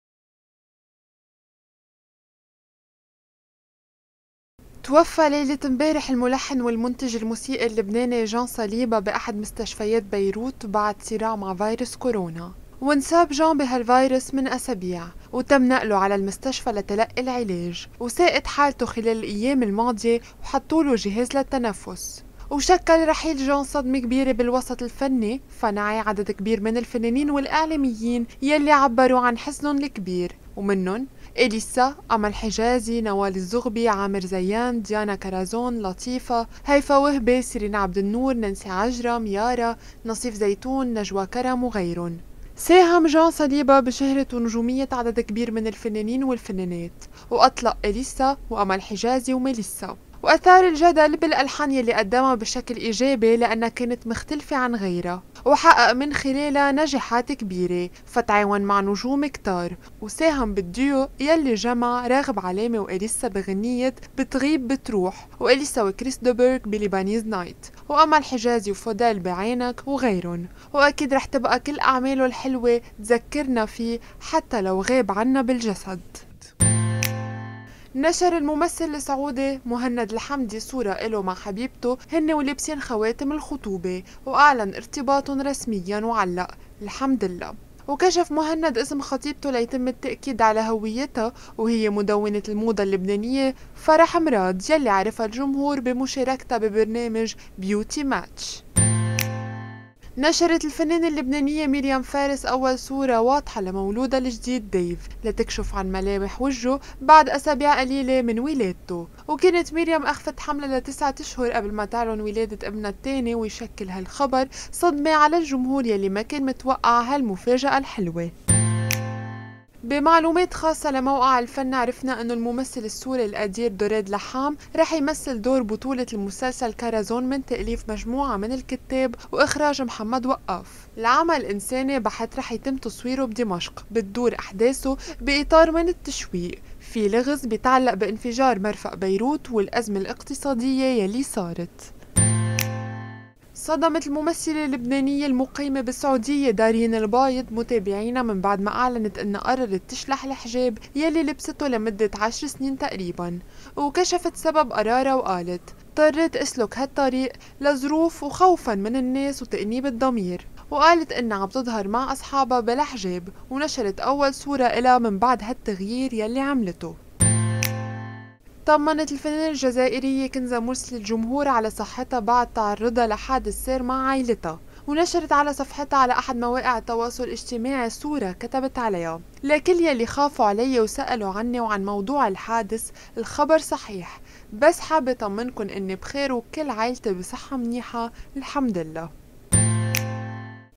الوسط الفني يفجع بوفاة جان صليبا.. ممثلة شهيرة تخلع حجابها وما علاقة دريد لحام بانفجار مرفأ بيروت؟
جان صليبا أطلق إليسا وأمل حجازي ولحّن لأهم الفنانين.. ورحيله بـ كورونا صدم الوسط الفني
مهند الحمدي يعلن خطوبته ويكشف عن هوية خطيبته اللبنانية!
خاص الفن – دريد لحام وإنفجار مرفأ بيروت في مسلسل واحد
ممثلة شهيرة تخلع الحجاب بعد ارتدائه 10 سنوات وتنشر صورتها لأول مرة من دونه
كنزة مرسلي تطمئن الجمهور بعد تعرضها لحادث سير برفقة عائلتها

Copyright : ElFann.com